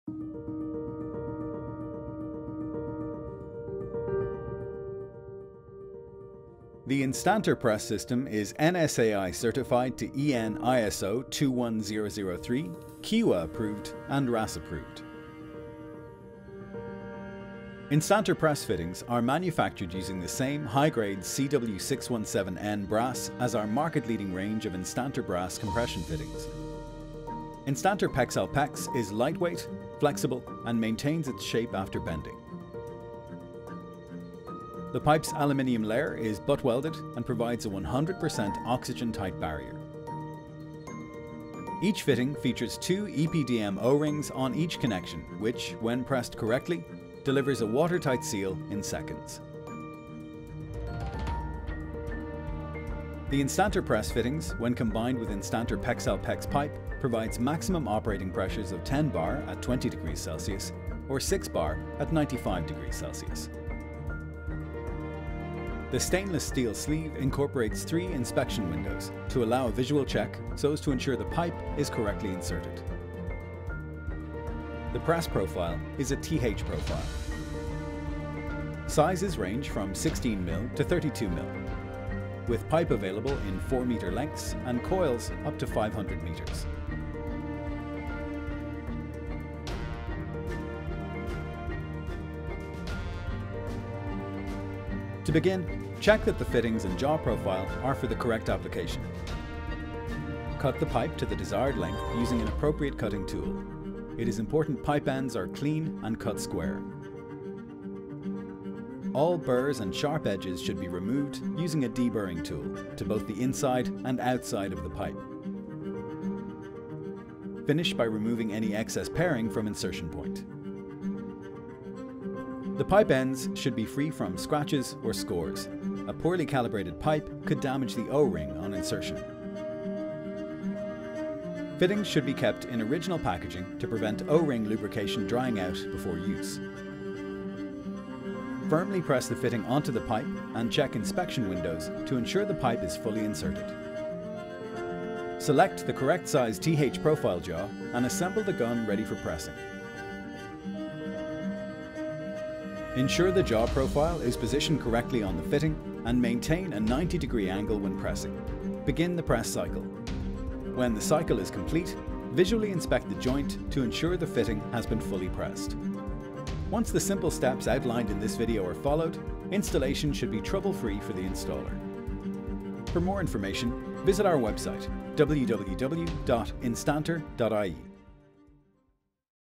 Instantor Press System
Instantor® Press fittings are manufactured using the same high-grade CW617N brass as our market leading range of INSTANTOR® Brass Compression fittings. Every fitting features two EPDM O rings on each connection which, when pressed correctly, delivers a water-tight seal in seconds.

Quick to install in three simple steps – Cut, Calibrate & Press, saving on time and costs as less fittings are required when compared with traditional plumbing methods. INSTANTOR® Press fittings are designed for use in sanitary water supply and heating systems.

Instantor Press Fittings are NSAI Certified to EN ISO 21003 and WRAS approved.

Watch our video for the full overview on Instantor Press Fittings.